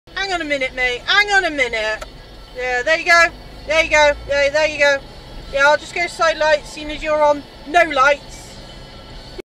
What you use your day lights
He was on day lights, so went to side light's
Day lights only work at that the front not the back of your car